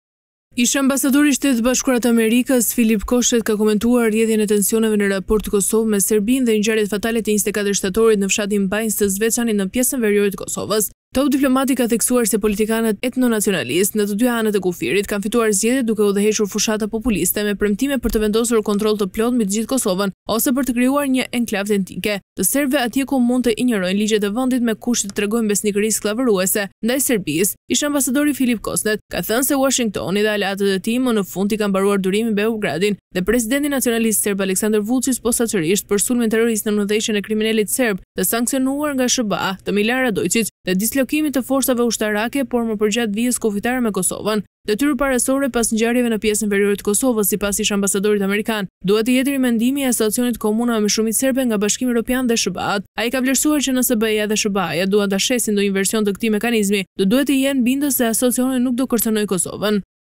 Kosnett: SHBA-ve më në fund i ka humbur durimi me Serbinë
Ish-ambasadori i Shteteve të Bashkuara të Amerikës, Philip Kosnett, ka komentuar rrjedhën e tensionuar në raportet Kosovë-Serbi dhe ngjarjet fatale të 24 shtatorit në fshatin Banjskë të Zveçanit në pjesën veriore të Kosovës.
Top diplomati ka theksuar se “politikanët etnonacionalistë” në të dy anët e kufirit, kanë fituar zgjedhjet duke udhëhequr “fushata populiste” me premtime për të vendosur kontroll të plotë mbi të gjithë Kosovën ose për të krijuar një “enklavë etnike” të serbëve atje, ku ata mund të injorojnë ligjet e vendit, me kusht që të tregojnë “besnikëri skllavëruese” ndaj Serbisë.
Ish-ambasadori Philip Kosnett, ka thënë se “Uashingtonit dhe aleatëve të tij më në fund i ka mbaruar durimi me Beogradin” dhe presidentin nacionalist serb Aleksandër Vuçiç, posaçërisht pas sulmit terrorist nën udhëheqjen e kriminelit serb të sanksionuar nga SHBA-të Millan Radoiçiç dhe dislokimit të forcave ushtarake serbe përgjatë vijës kufitare me Kosovës.
Detyrë parësore pas ngjarjeve në pjesën veriore të Kosovës, sipas ish-ambasadorit amerikan, duhet të jetë “rimendimi” i Asociacionit të Komunave me shumicë serbe nga Bashkimi Evropian dhe SHBA-të. Ai ka vlerësuar që nëse BE-ja dhe SHBA-të duan ta “shesin” ndonjë version të këtij mekanizmi, do t’u duhet të jenë bindës se asociacioni nuk do të kërcënojë Kosovën.
“Kosovarët kanë frikë se Asociacioni i Komunave me shumicë serbe mund të përbëjë një kërcënim të brendshëm të sigurisë dhe të parashikojë ndarje de facto. Shumë e krahasojnë Asociacionin e Komunave me shumicë serbe me Republikën Srpska ultranacionaliste të Bosnjë-Hercegovinës”, ka kujtuar ndër të tjera Kosnett.

𝐀𝐮𝐭𝐨𝐫 : NTV